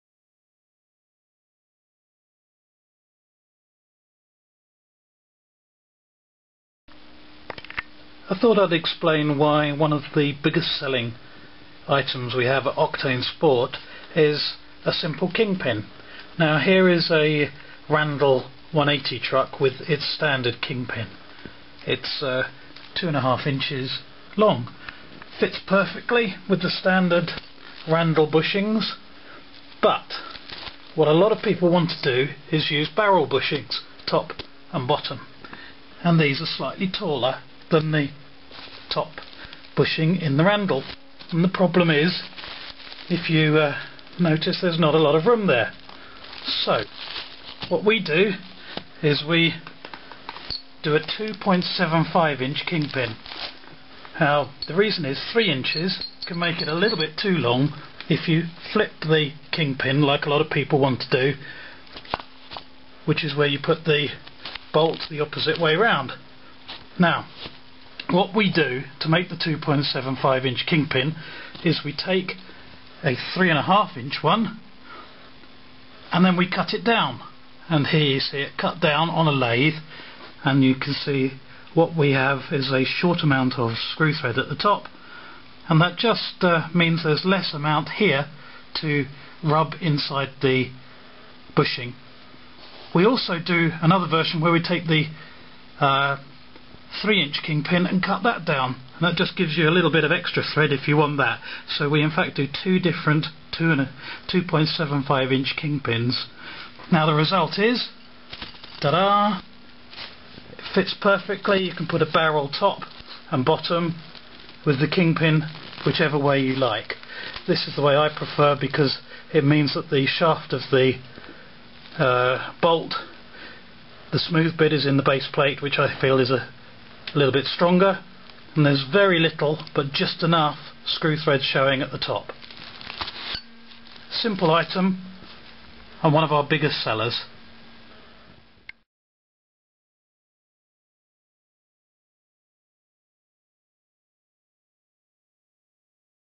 Randal Longboard Truck Kingpin upgrade to 2.75" Grade 8
Our custom Grade 8 (US) kingpin, cut down from 3.5" or 3" bolts to make a perfect fit for Randal trucks with barrel bushings top and bottom.